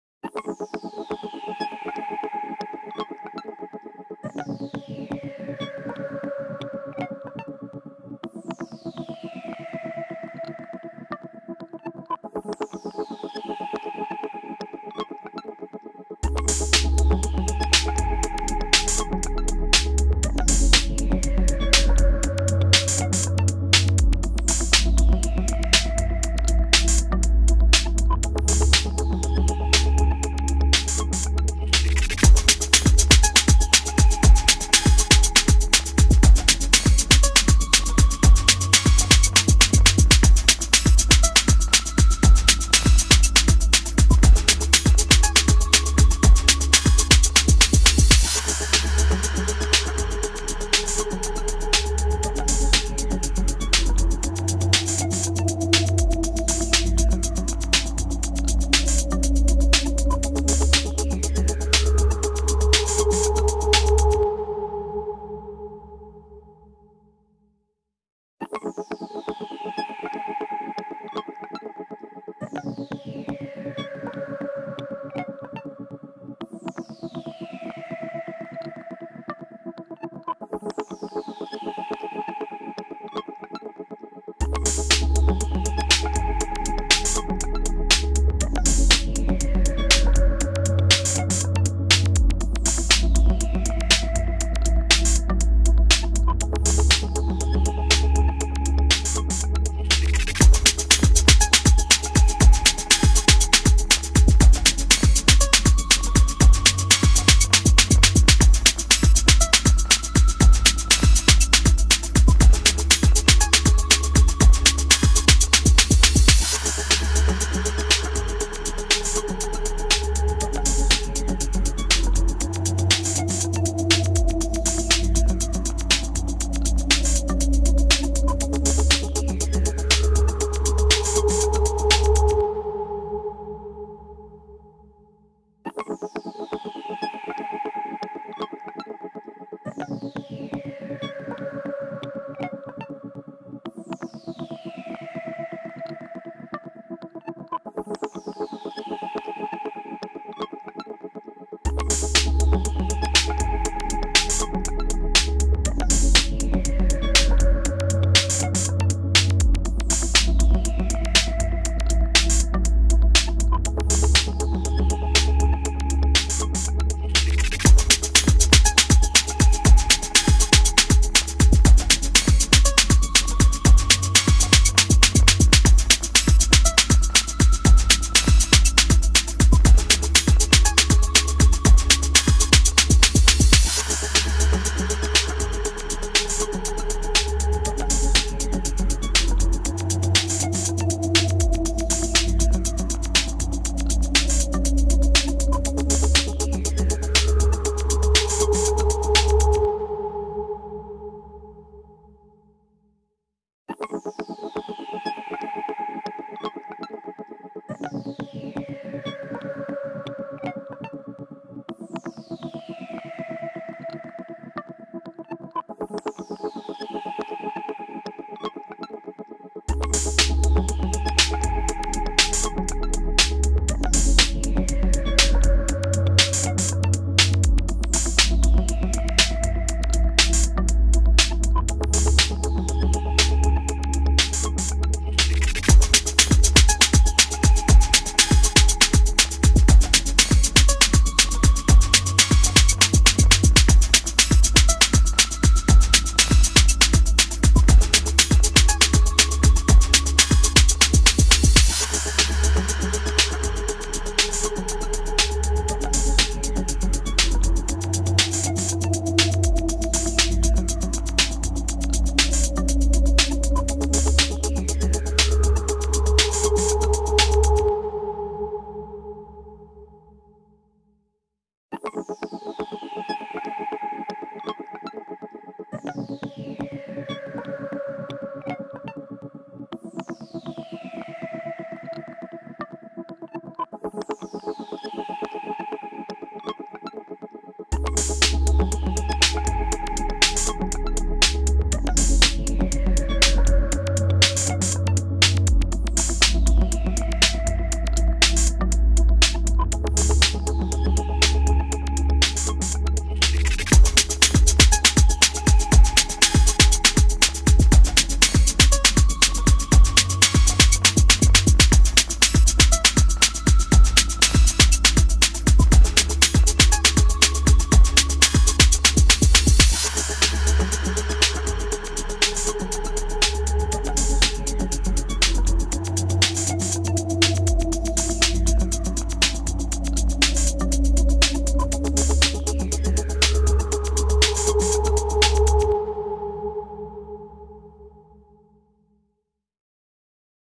edge of night is broken...
i dont usually record much so missedthe chance to capture how i got early 4 edge of night.